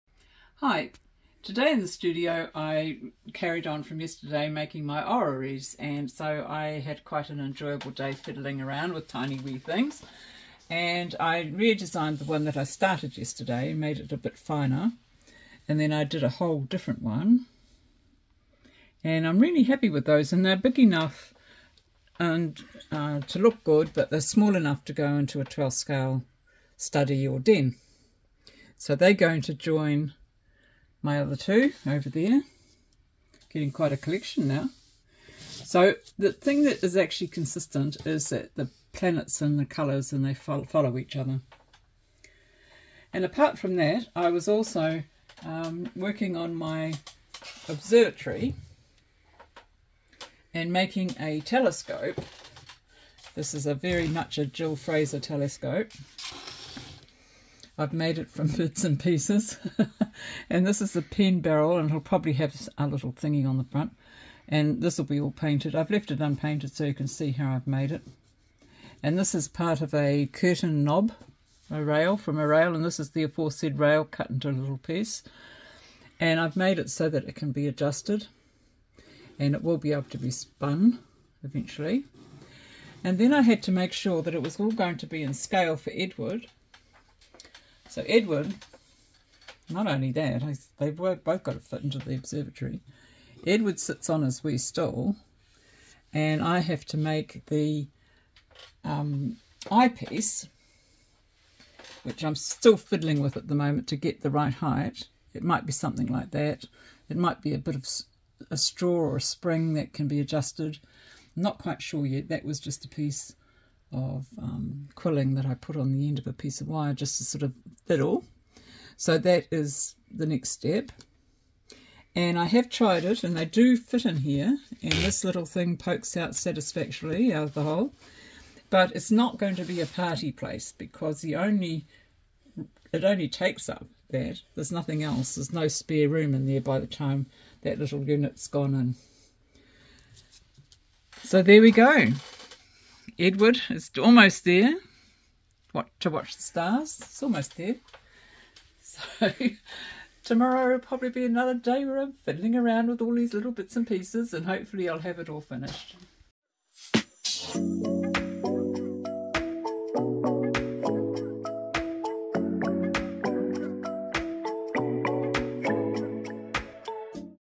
Day 93: I finished the Miniature Orreries and Built a Miniature Telescope
On Day 93 of 'A Year in the Life of a Miniaturist' I finished the miniature orreries that I was making yesterday, and built a prototype 12th scale telescope. I think it's getting there and will probably be able to be installed in the observatory tomorrow. Who knows what Sam and Amelia may get up to while Edward is star-gazing though?!